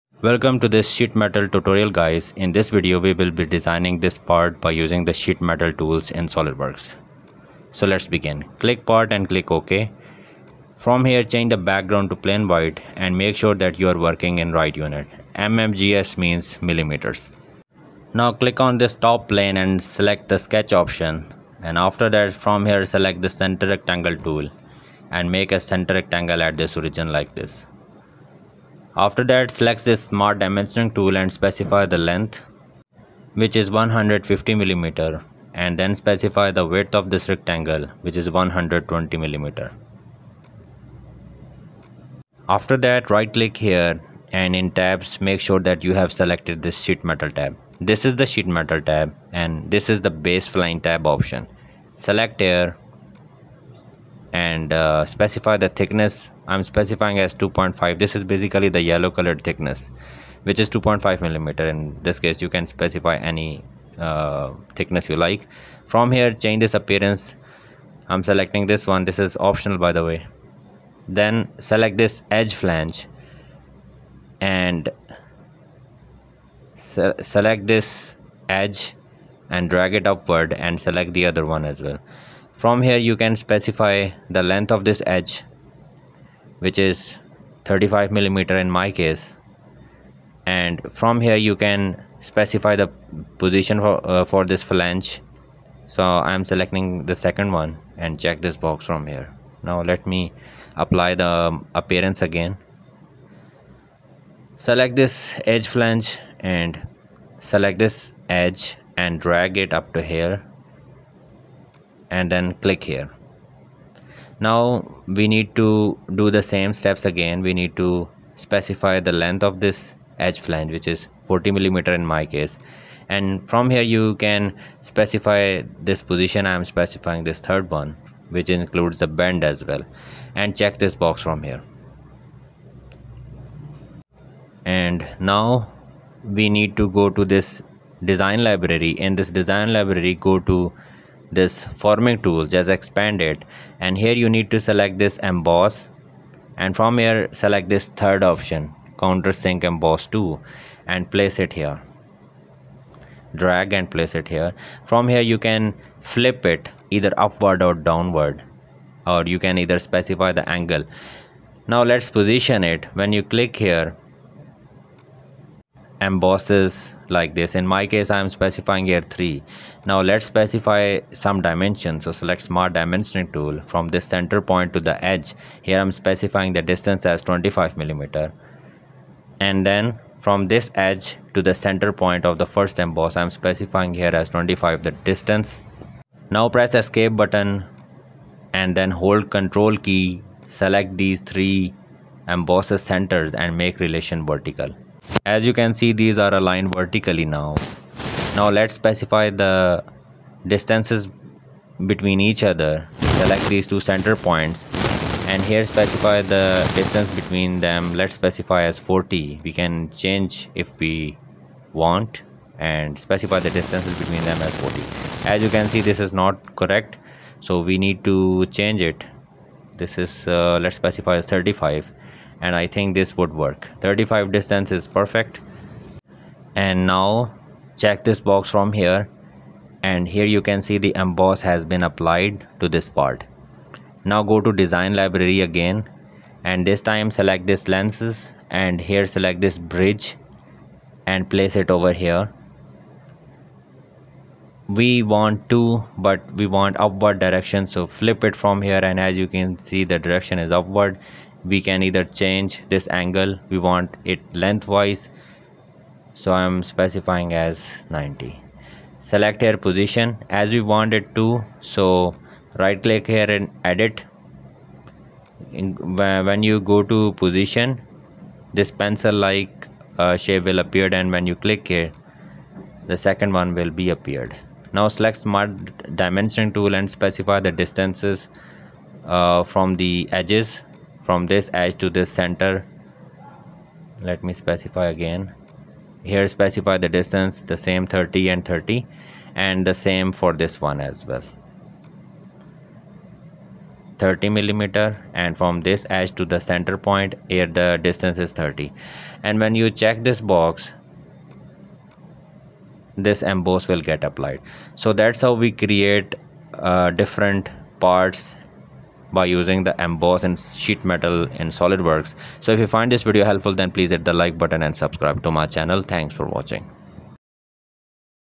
Sheet Metal SOLIDWORKS Tutorial Emboss | Robo CAD
in this sheet metal solidworks tutorial, a part is designed using sheet metal tools available in SolidWorks. This tutorial is more focused on Forming tools or Ebmoss tools.